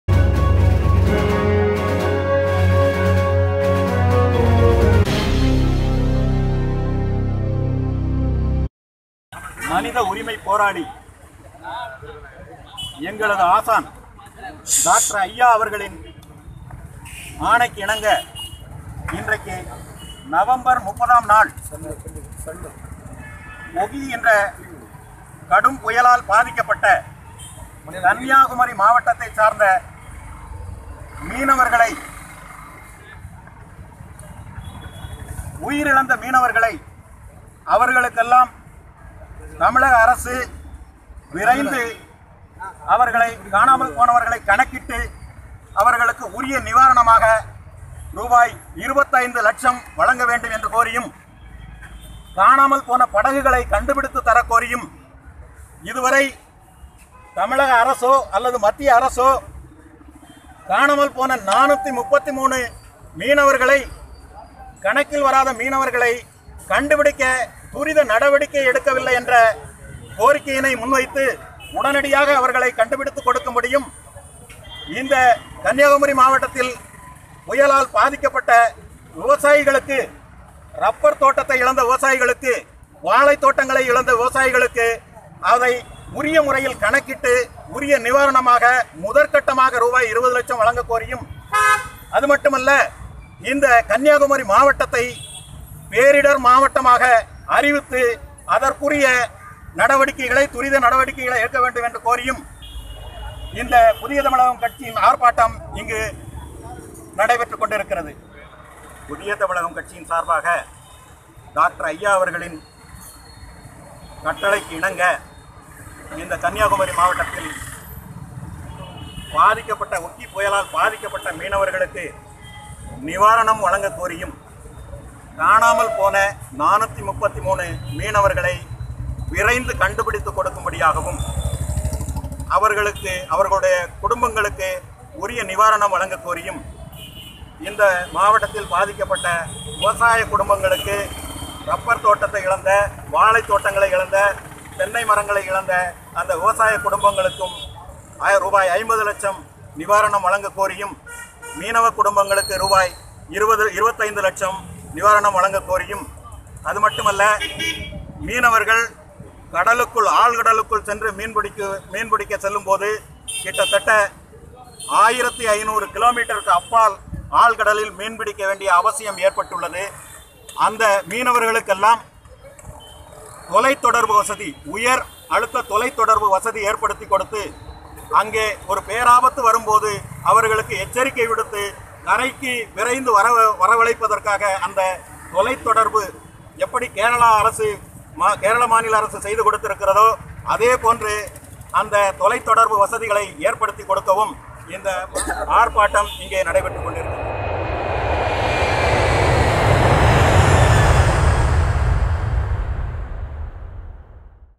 கன்னியாகுமரி மாவட்டத்தை பேரிடர் மாவட்டமாக அறிவிக்க வேண்டும்- VK அய்யர்.
ஒக்கி புயலில் பாதித்த மீனவர்களுக்கு உரிய நிவாரணம் வழங்கக்கோரி  புதிய தமிழகம் கட்சி ஆர்ப்பாட்டம்,,,